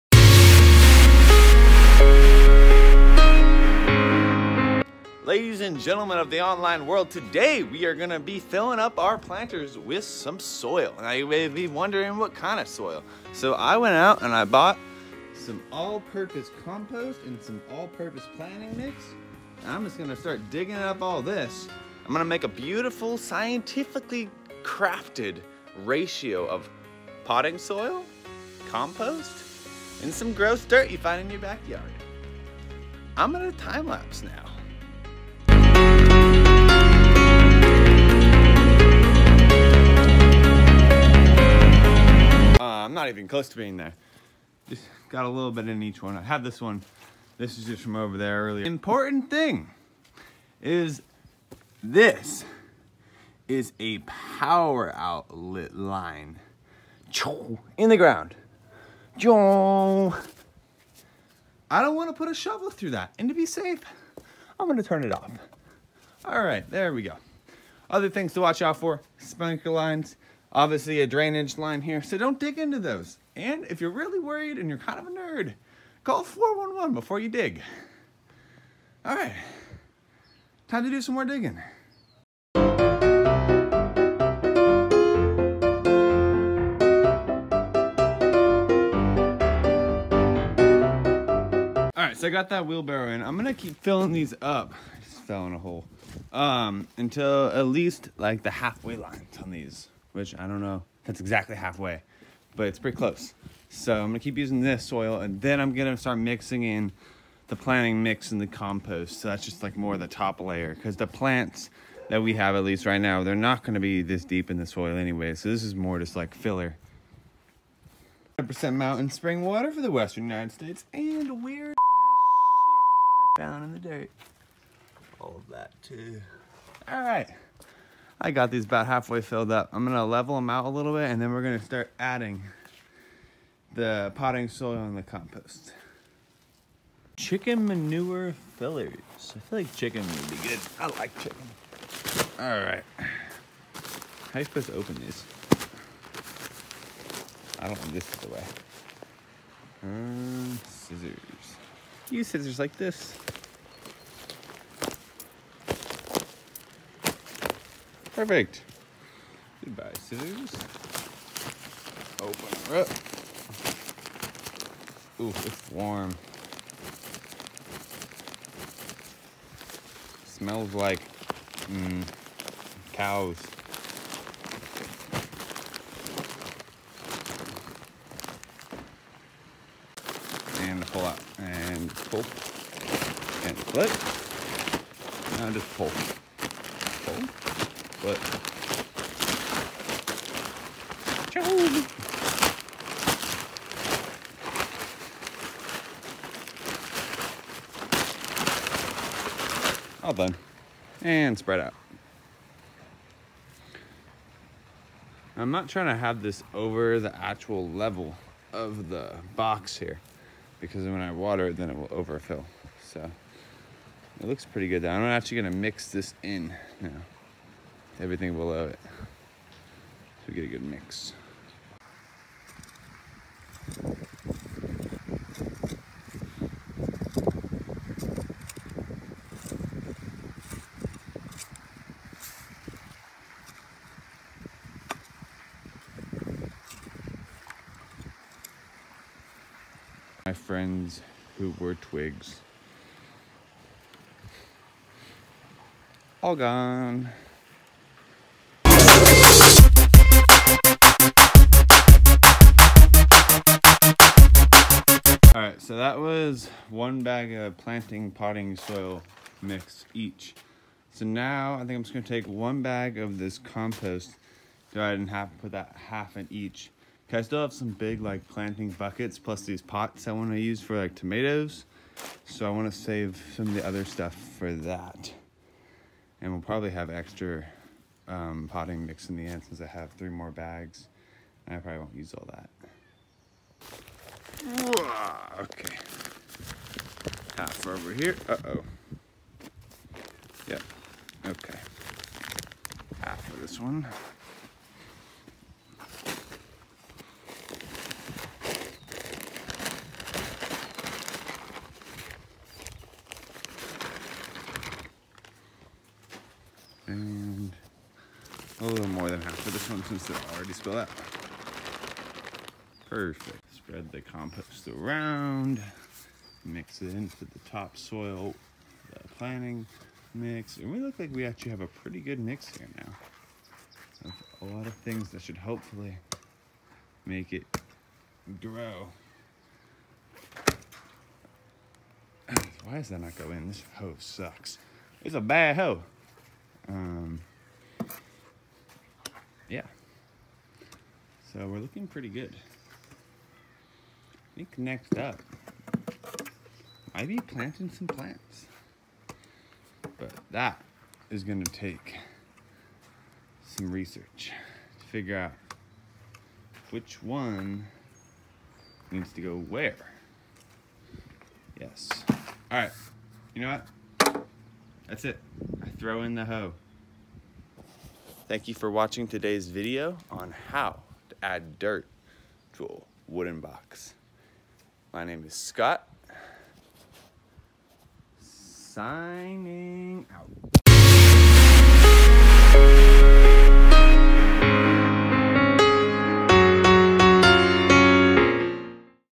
HOW TO MAKE GARDEN SOIL MIXTURE
Before we can plant the raised planter beds we need the proper soil. I am going to show you today how to create the proper ratio of top soil, compost, and potting mix. (From my previous channel Scott Builds)